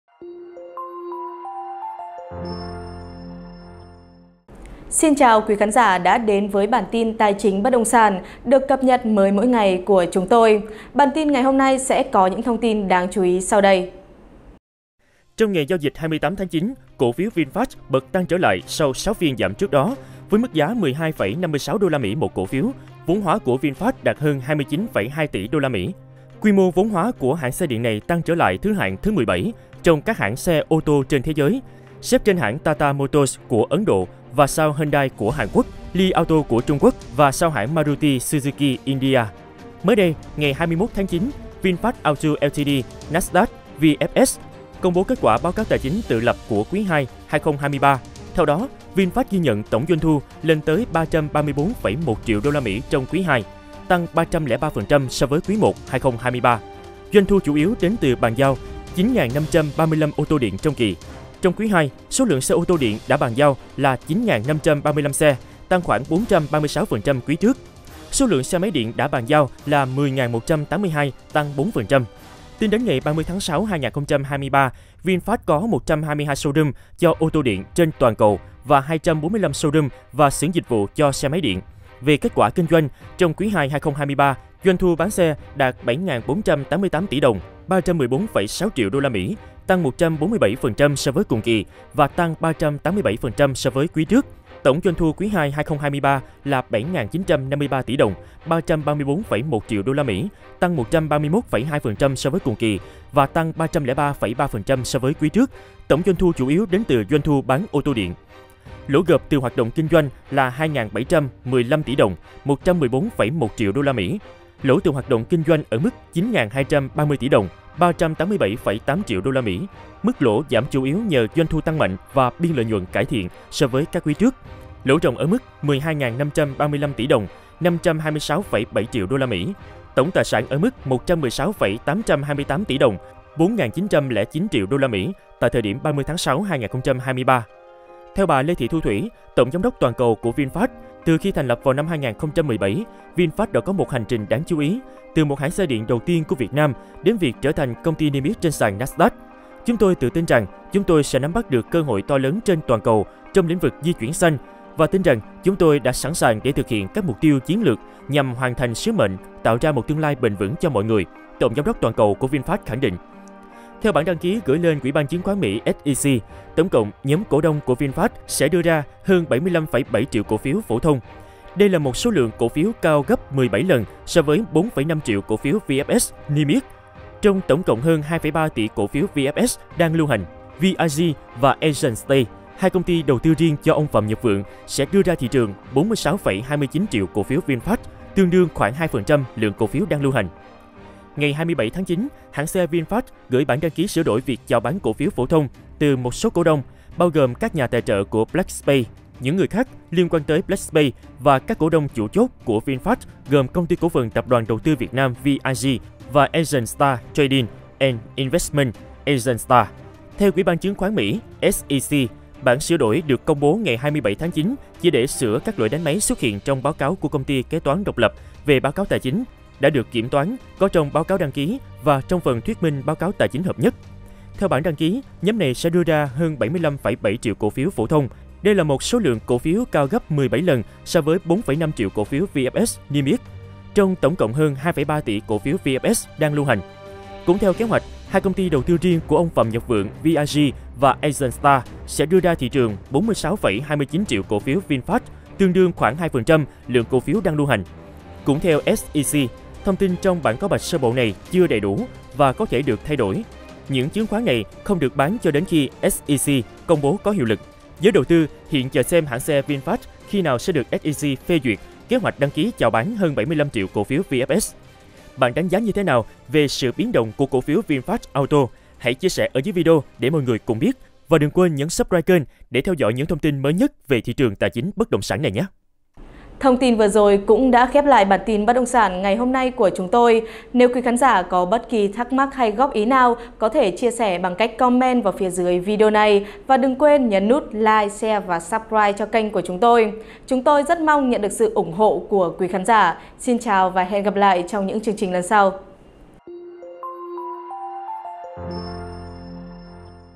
Bất ngờ: Cổ phiếu VinFast BẬT TĂNG trên sàn Nasdaq, vốn hóa đạt hơn 29 tỷ USD | CafeLand
Trong ngày giao dịch 28/9, cổ phiếu VinFast bật tăng trở lại sau 6 phiên giảm trước đó. Với mức giá 12,56 USD/cổ phiếu, vốn hóa của VinFast đạt hơn 29,2 tỷ USD.